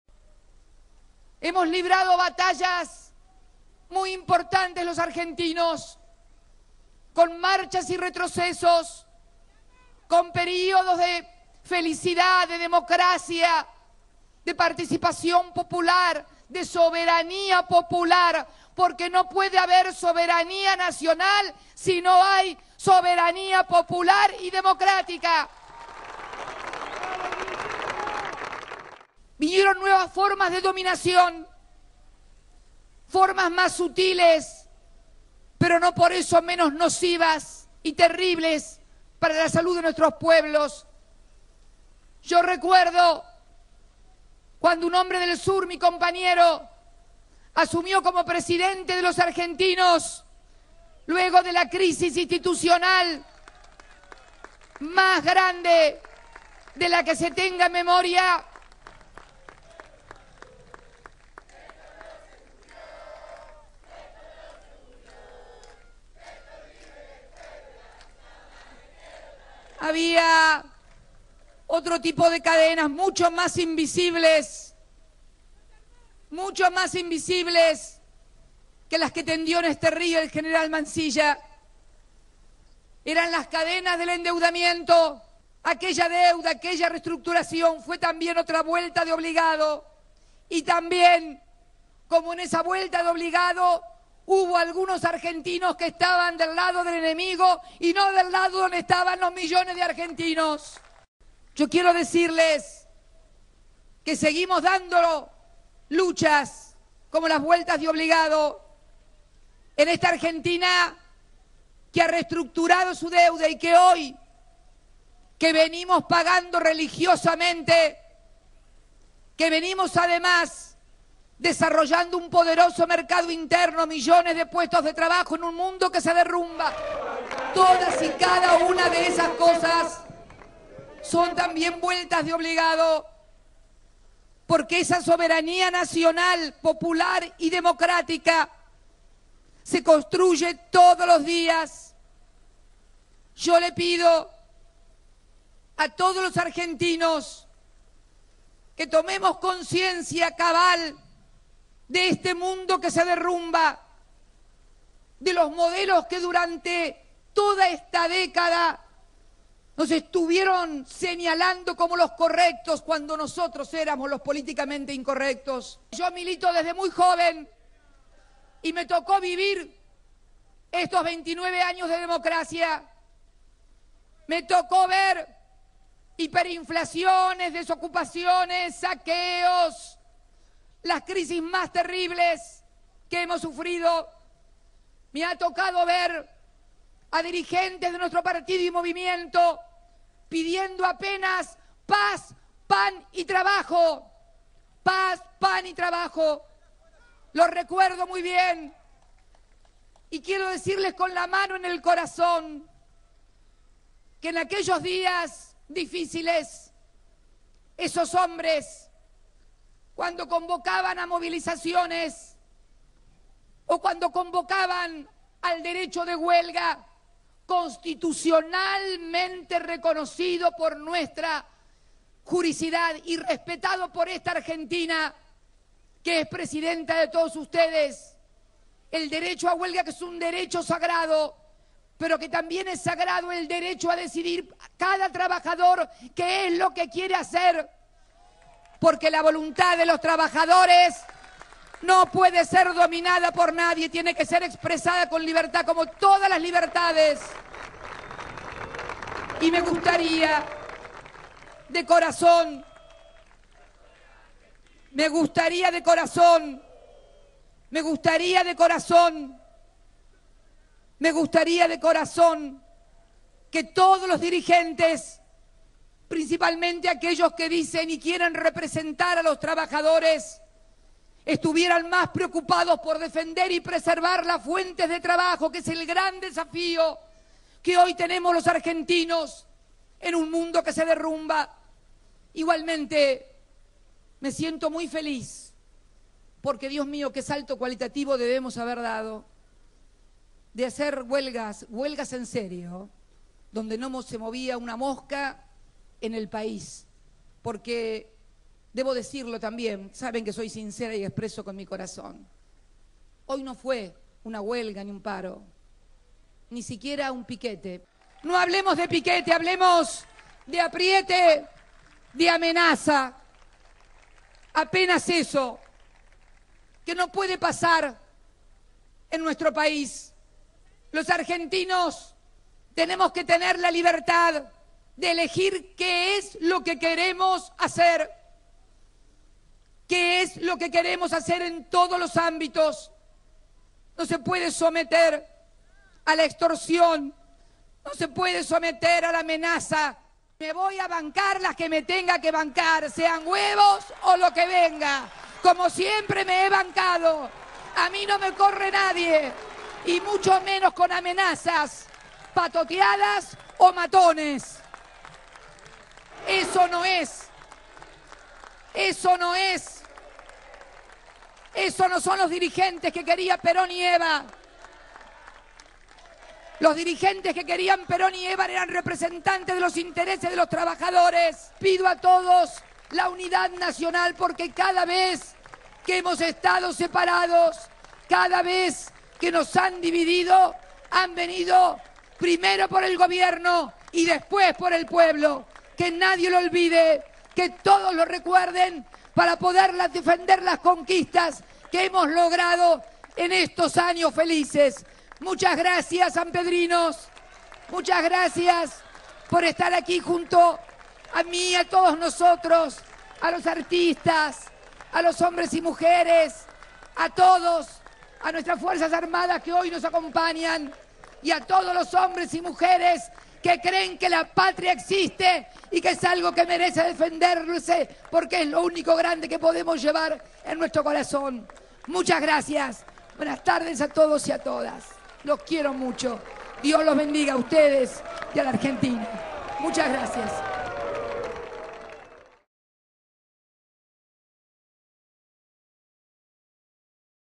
"A mí no me corren con amenazas o matones", dijo la Presidenta.
La presidenta Cristina Fernández de Kirchner advirtió hoy que "me voy a bancar las que me tenga bancar, porque a mí no me corre nadie, y menos con amenazas o matones", porque "esos no son los dirigentes que querían Eva y Perón", y agregó que el paro de hoy "no fue un piquete, fue un apriete y una amenaza".
La jefa de Estado, al hacer referencia al paro convocado por las centrales disidentes de la CTA y la CGT, expresó además que "el derecho a huelga es sagrado, pero también es sagrado que el trabajador decida que quiere hacer", y agregó que "la voluntad de los trabajadores no puede ser dominada por nadie".
Al hablar en San Pedro, durante el acto por el Día de la Soberanía Nacional, Cristina se pronunció en contra del "corte, la amenaza o la presión", porque "la gente quiere laburar y poder decidir libremente".
Esas fueron las expresiones de la Presidenta al referirse esta tarde a lo que las centrales obreras disidentes calificaron de "paro", pero que según la mandataria fue una "apriete" dado que no se permitió a los argentinos que querían trabajar que pudieran hacerlo libremente.
 En ese contexto, la presidenta enfatizó: "Los argentinos tenemos que tener libertad de elegir en todos los ámbitos".
Por otra parte, la jefa de Estado también pidió a los dirigentes sindicales que estén "más preocupados por defender las fuentes de trabajo que es el gran desafío que tenemos los argentinos en un mundo que se derrumba".